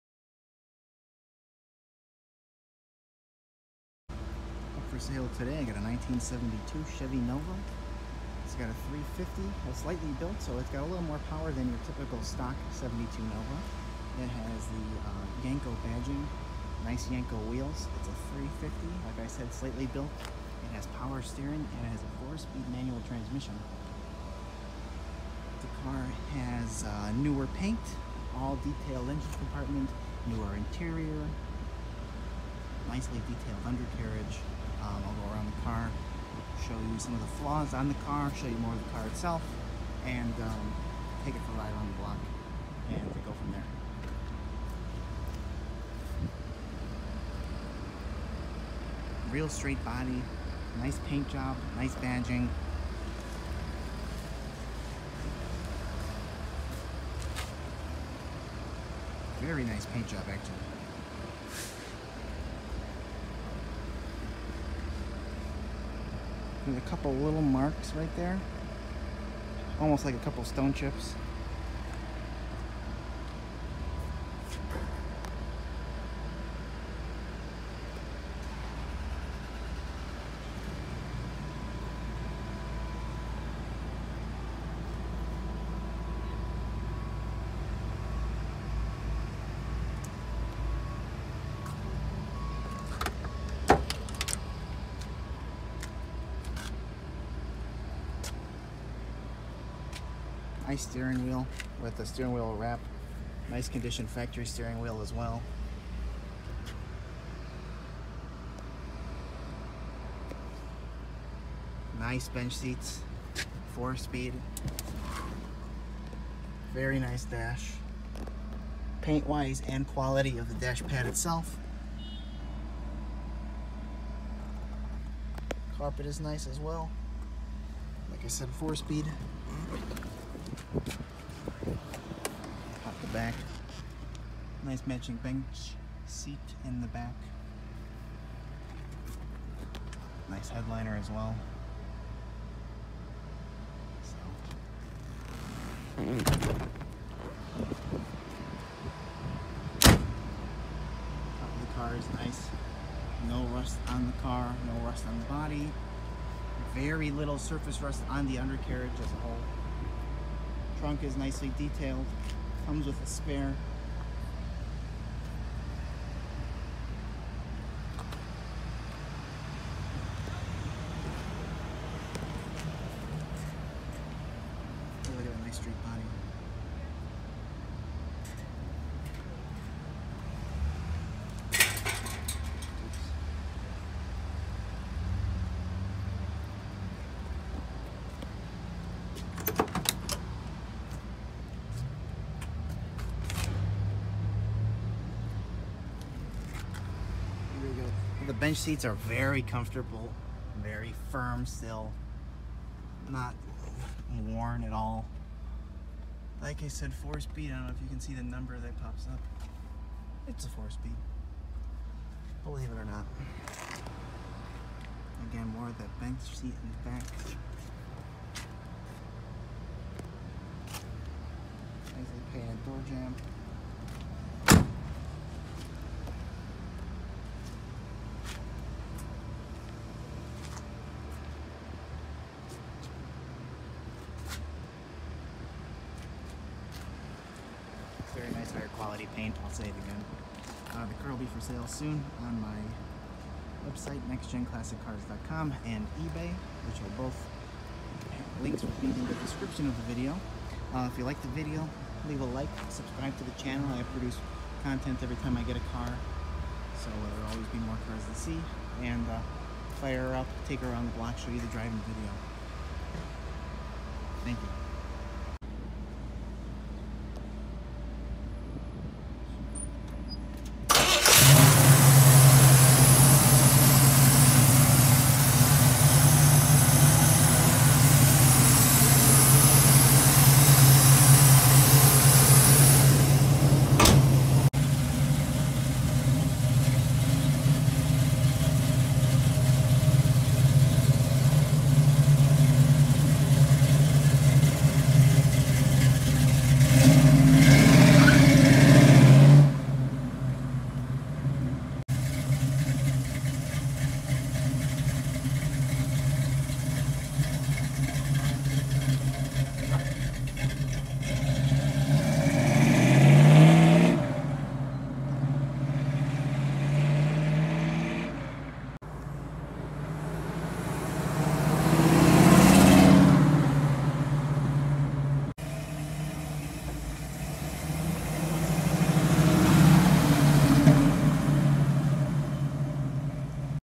1972 Chevrolet Nova - 4spd - Yenko Clone - SOLD
1972 Chevrolet Nova For Sale! 4 Speed Manual Transmission! 350 slightly Built! Power Steering! Yenko Clone Style With New Interior! Very Nice And Detailed In And Out!